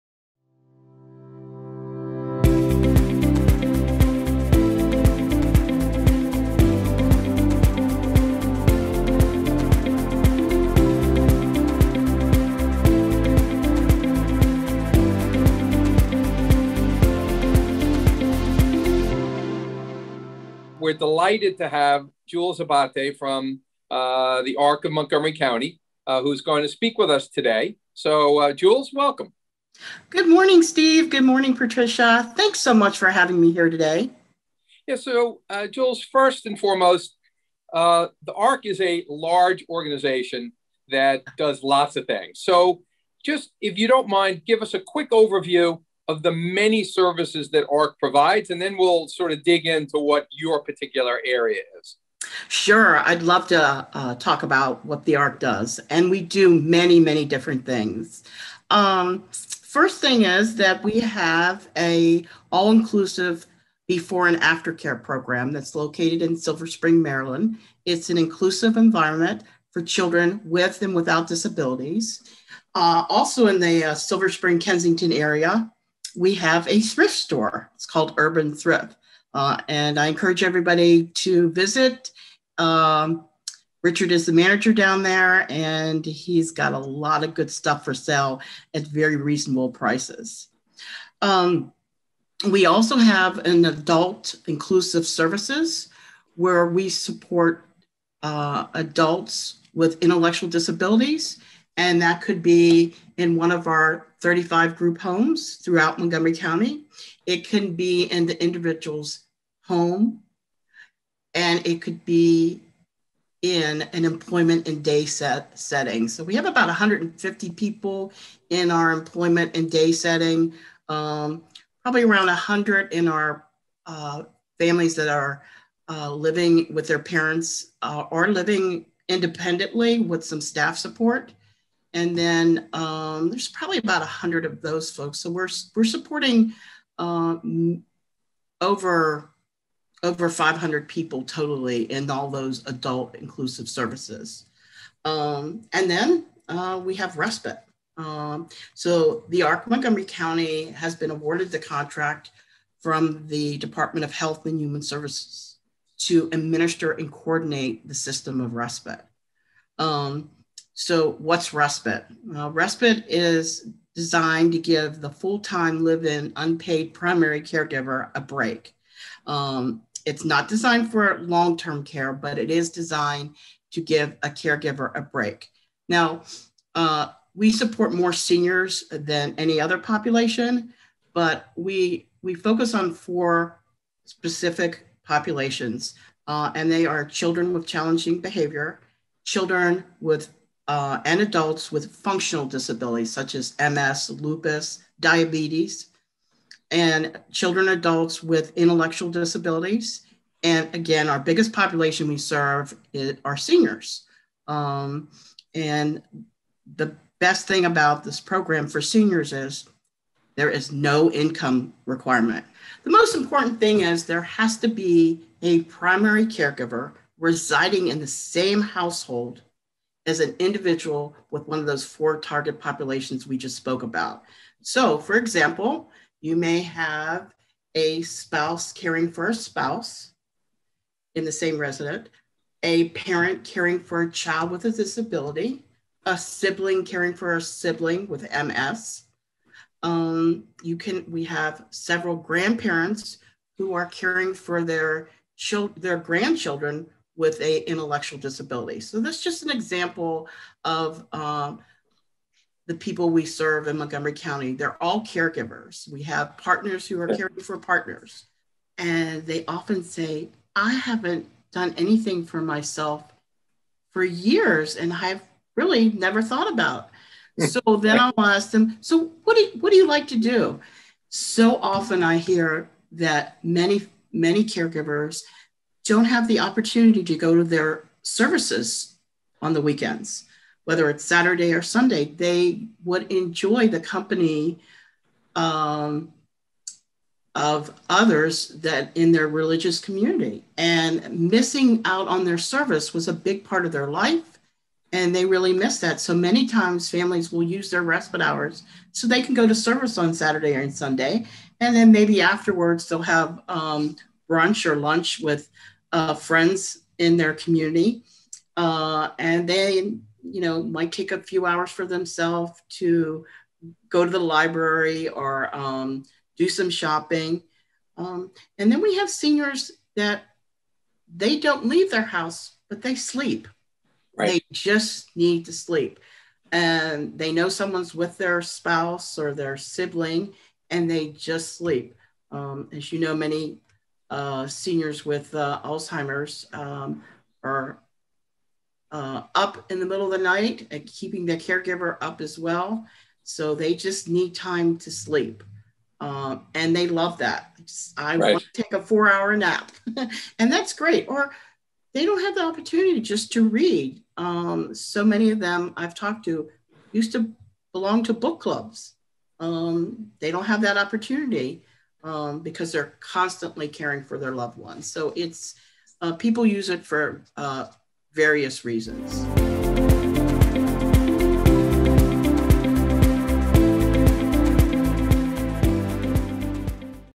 The Arc of Montgomery County
Learn about the ARC of Montgomery County and respite care services.